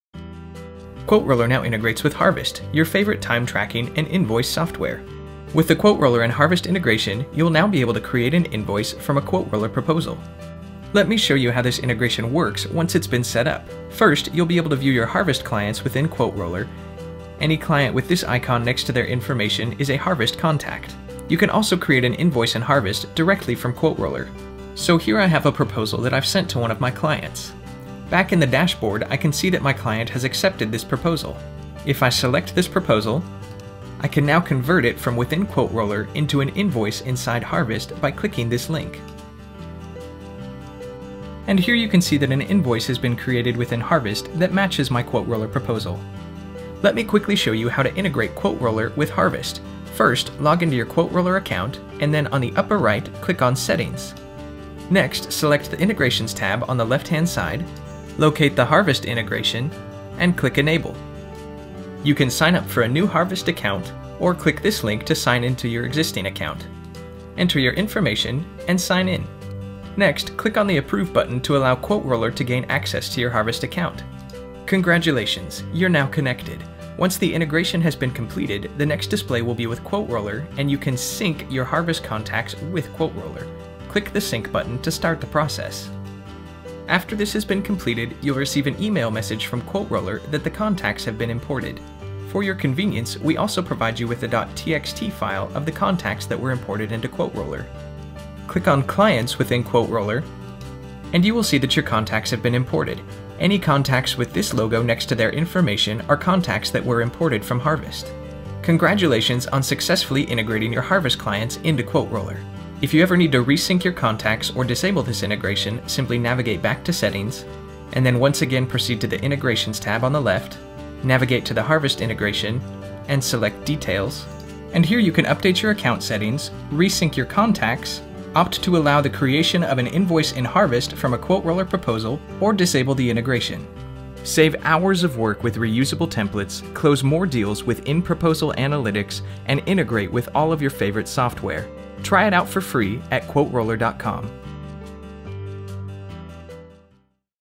Quote Roller and Harvest Integration Video
Close more deals with Quote Roller & then invoice them with Harvest
Bill by the hour and close in minutes proposal software and Harvest project manager. Quote Roller lets you offer your clients a flexible, easy-to-make pricing table with hourly and quantity options. Then, Harvest turns that into an easy-to-sign and pay invoice.